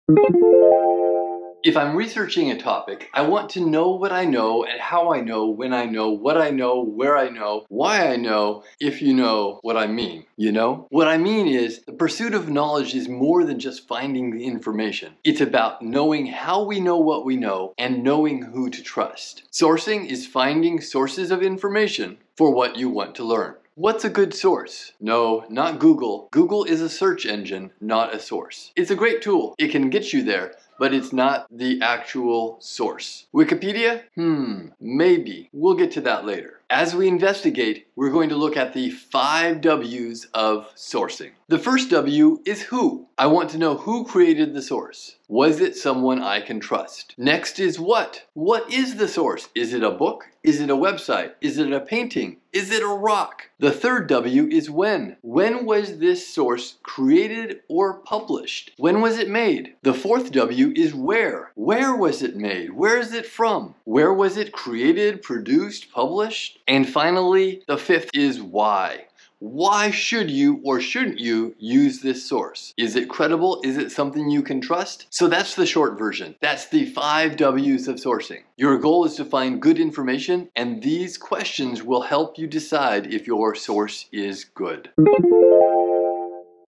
The Five W's of Sourcing: Just the Basics
Show Transcript December 12, 2021

If I'm researching a topic, I want to know that I know what I know when I know what I know. You know?

What I mean is that the pursuit of knowledge is more than just finding information. It's knowing how we know what we know. Is about who and what to trust.

When we research, we look for sources of information. We call this sourcing. Sourcing is finding sources of information for what you want to learn.
What's a good source?
No, not Google. That's a search engine, not a source.
Wikipedia? Hmm. Let's look at that one later.

As we investigate, we'll look for the five ws of sourcing.
Who
The first W is Who.
I want to know who created the source. Was it someone I can trust?
What
The second W is What. What type of source is it? Is it a book or a website? Is it a video or a sculpture?
When
The third W is When. When was it created? When was it made?
Where
The fourth W is Where. Where was it created, produced or published?
Why
Finally, the fifth W is Why. Why do you want to use this source, or why shouldn't you use this source?

So that's the short version. The basics. Your goal is finding good information and these questions will help you decide if the information is good.